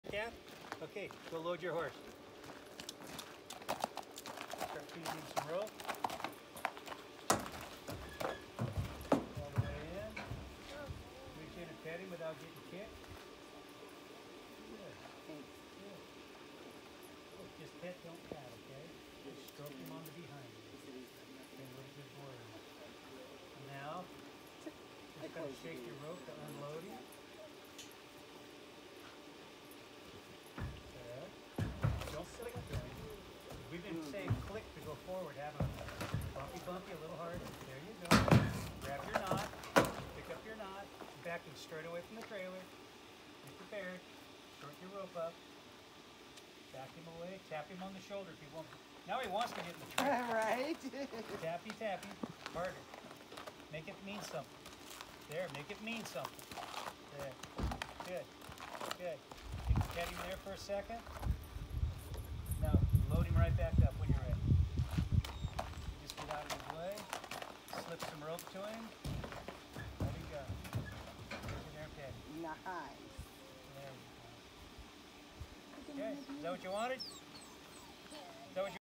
Trailer Loading
The story we got is they got the horse in April, they fought to get him in for delivery home, took a lot of drama, then haven't been able to get him loaded since. Maybe get front feet to the ramp or maybe in then run backwards and refuse. So they called and asked if I could help and of course my canned answer is "all I can do is try". This was about 20 minutes later... We didn't film any of the start but Oh well. Trust me, there was no blow up, no running around, no sweat, no panic, just clear communication, instruction, correction and reward.